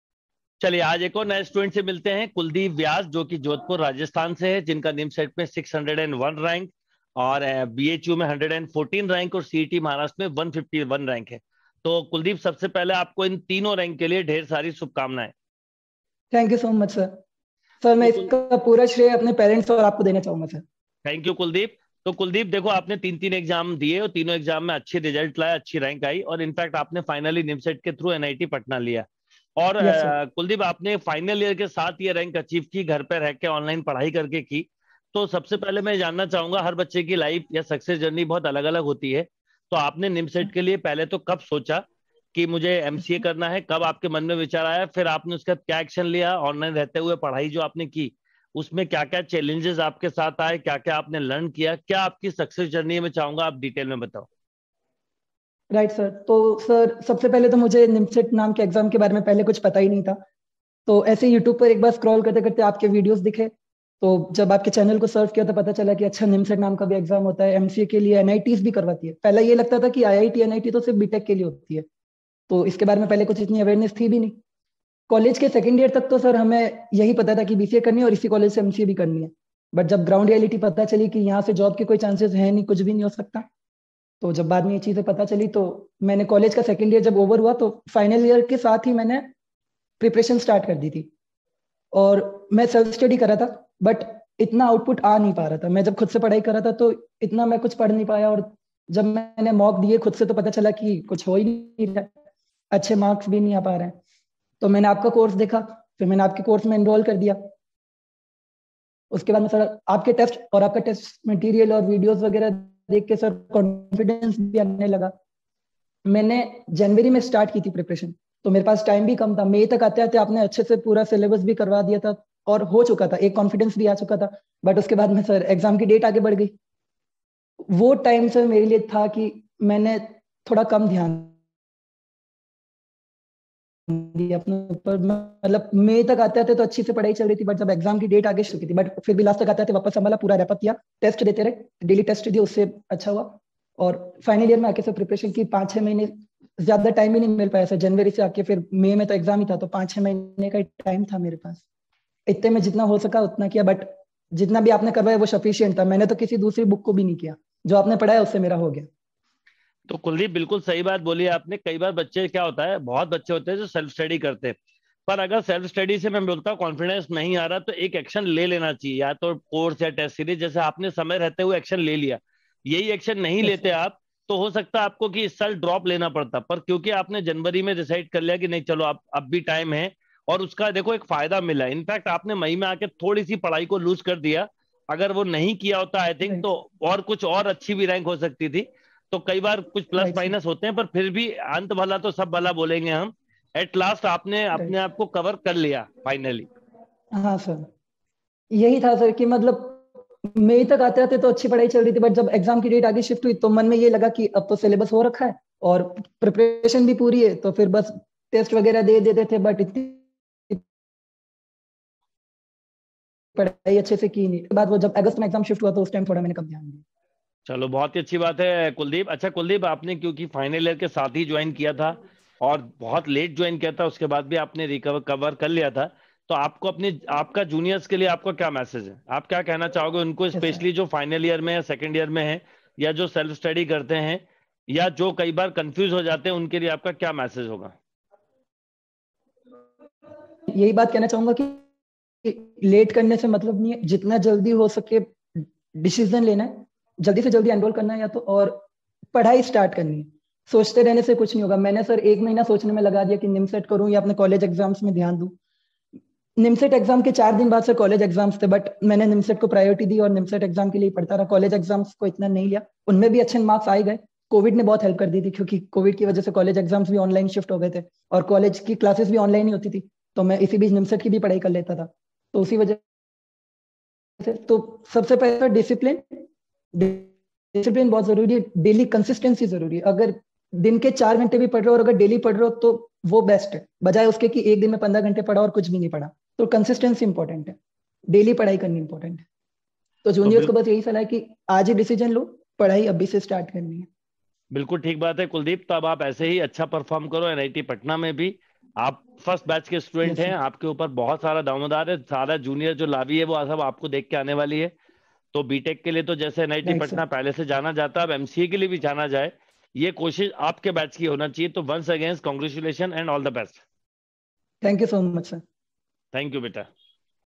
Winning strategy of "KULDEEP VYAS" NIMCET - 601 | BHU - 114 | MHT-CET - 151 | NIMCET - 21
This is Sharad Neha Gupta and in this video "KULDEEP VYAS" will share his experience of achieving AIR - 601 In NIMCET 2021 Exam. So sharing with you all his success journey with Impetus Gurukul.
Winning strategy of "KULDEEP VYAS" AIR - 601 NIMCET - 21 | Impetus Gurukul